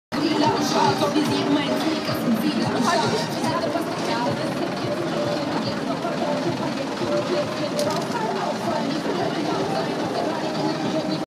Berlin - 8. März 2014 - Street Art
When Street Art Graffiti meets advertising and the reverse
Have a look at the Converse ad in the Press and you'll see...
What are this guy's walking shoes?